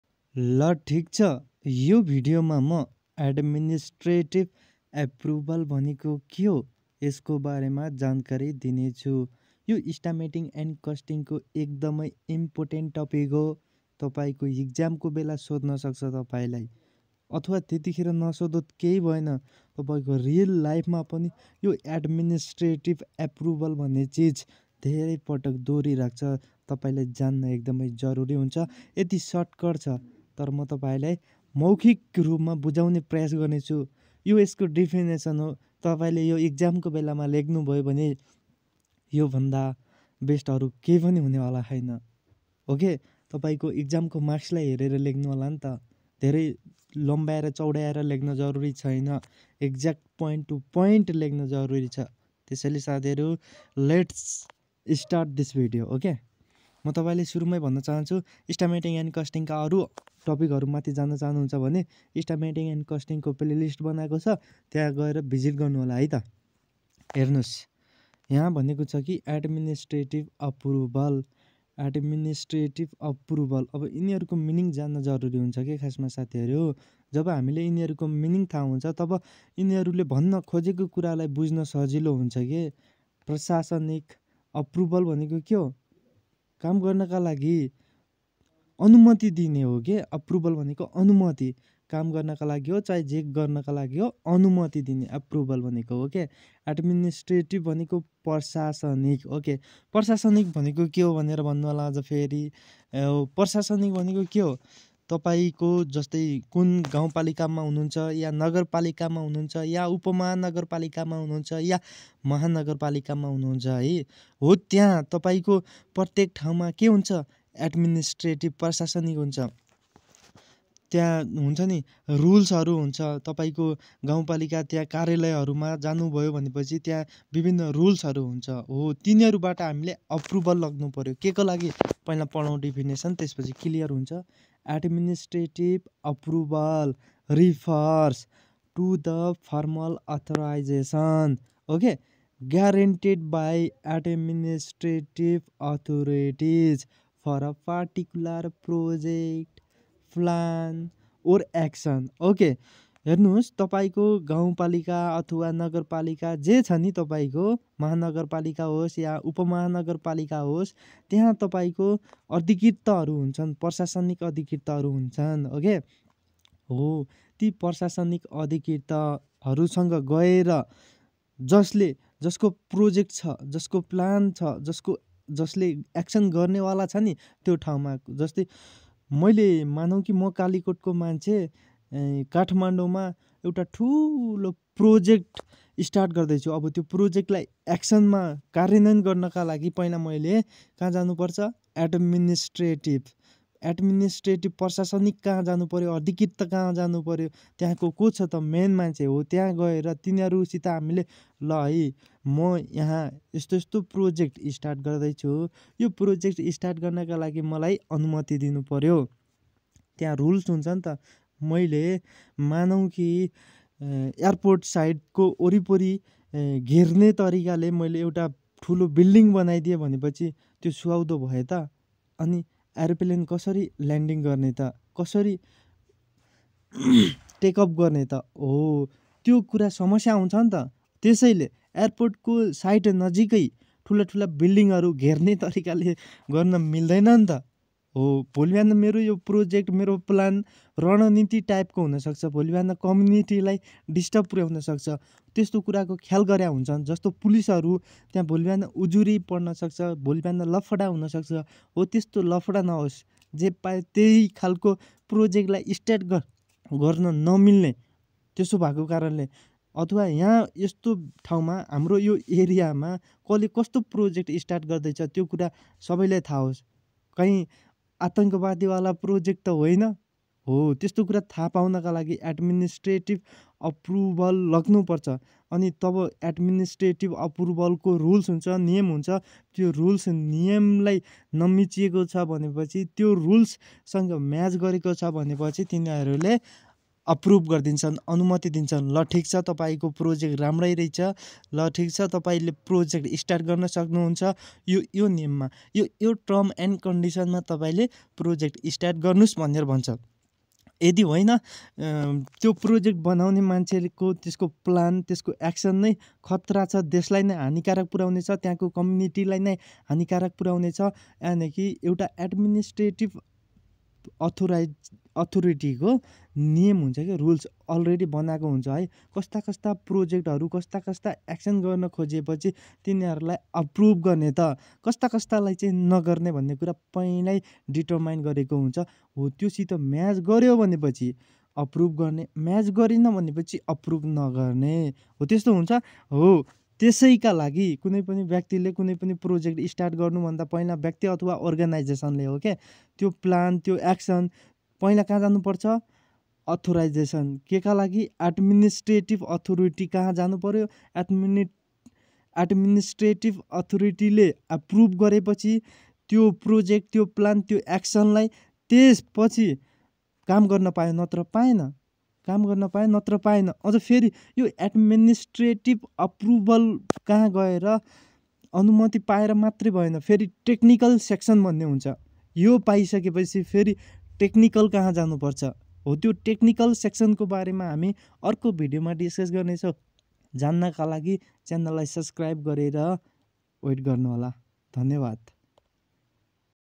Administrative Approval in Estimating and Costing in Nepali
Administrative Approval , Define Administrative Approval , Estimating and Costing in Nepali

"Demystifying Administrative Approval: Understanding Estimating and Costing in Nepali"

Estimating and Costing
Unit 1: Introduction :

In this informative video, we delve into the intricacies of Administrative Approval, define what it entails, and explore the fascinating realms of Estimating and Costing in Nepali context. Join us as we demystify the process of Administrative Approval and offer valuable insights into understanding the crucial aspects of Estimating and Costing in Nepal.

Administrative Approval plays a pivotal role in project management, ensuring compliance with relevant rules and regulations, and obtaining the necessary authorizations for project execution. By comprehending this vital step, individuals and organizations can streamline their projects, enhance efficiency, and avoid unnecessary hurdles.

Additionally, we will dissect the complexities of Estimating and Costing in Nepali, shedding light on the fundamental techniques, strategies, and considerations. Estimating accurately is crucial for successful project implementation, as it aids in budgeting, resource allocation, and overall project planning. Costing, on the other hand, involves determining the financial aspects of a project, ensuring it remains economically viable and sustainable.

Our aim is to provide viewers with comprehensive knowledge and actionable insights into Administrative Approval, Estimating, and Costing in the context of Nepal. Whether you are a student learning about these concepts or a professional seeking to enhance your understanding, this video will serve as a valuable resource.

So, join us on this educational journey, unravel the mysteries surrounding Administrative Approval, and gain a deeper comprehension of Estimating and Costing in the Nepali scenario. Don't miss out on this opportunity to expand your knowledge and embrace the intricacies of these essential project management elements.